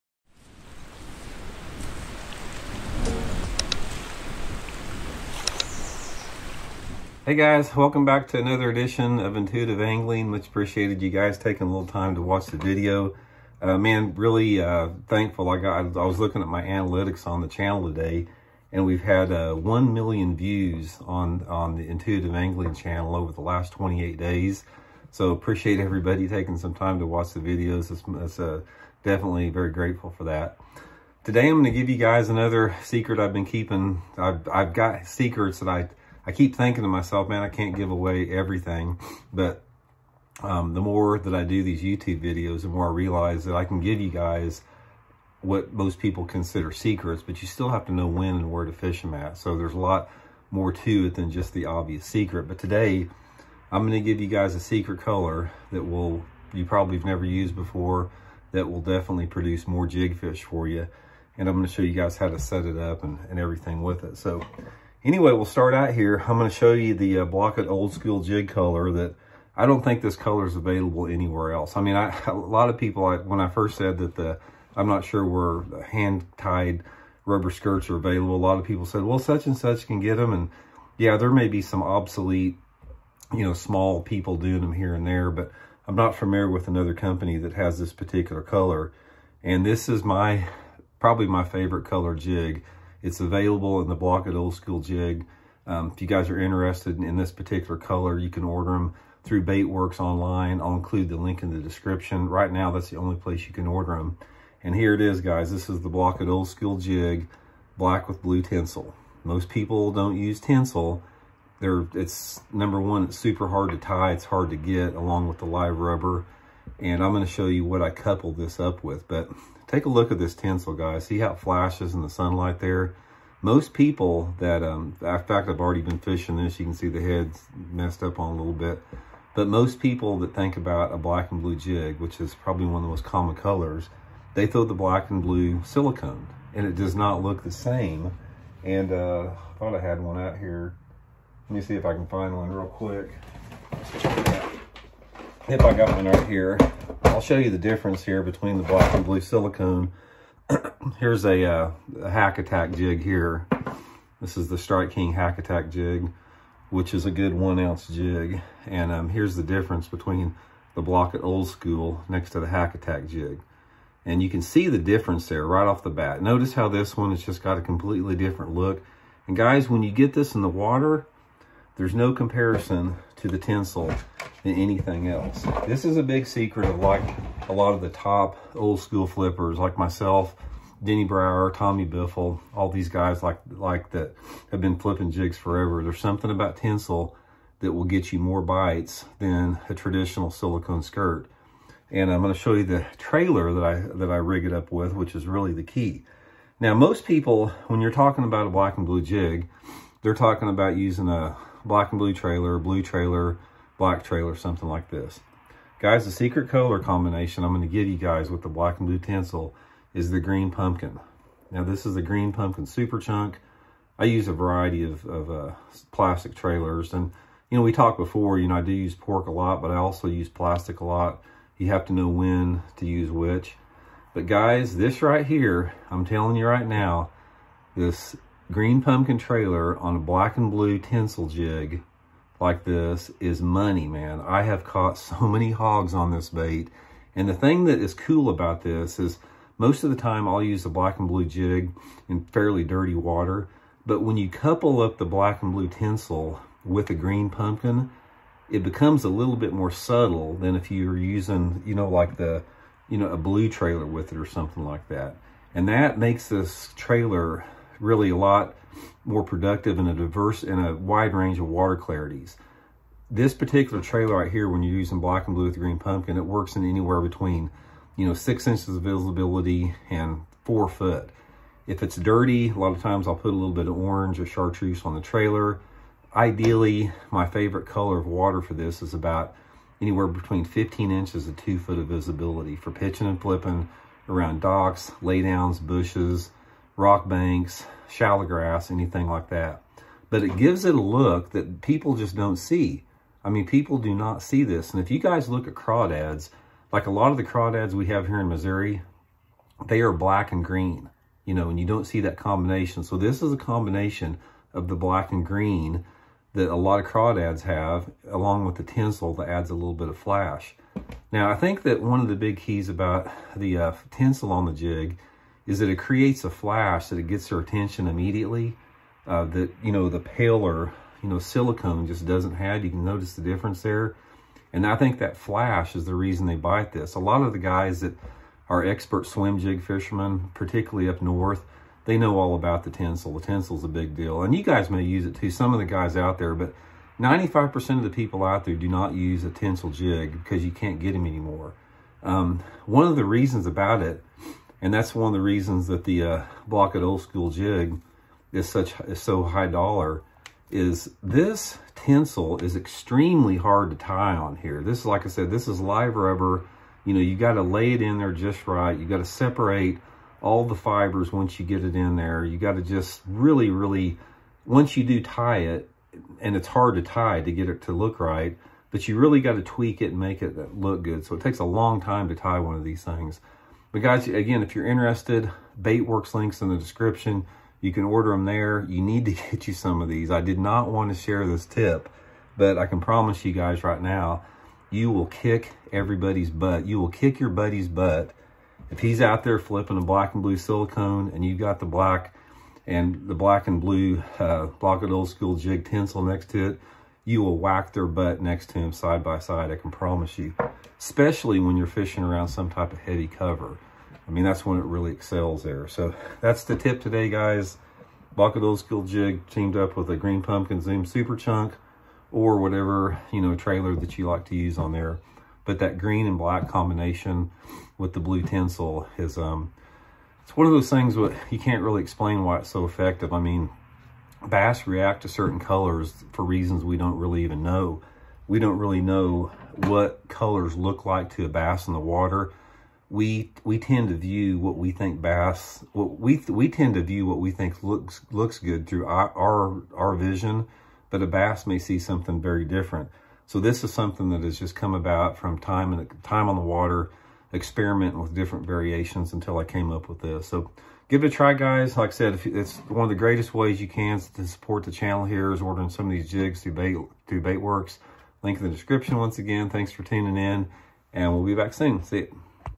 Here It Is…My Secret Jig Color Never Seen Before
Randy shares with everybody one of his secret jig colors that he’s kept from the public for over 40 years and gives tips and advice on how to fish it…blaukatoldschooljig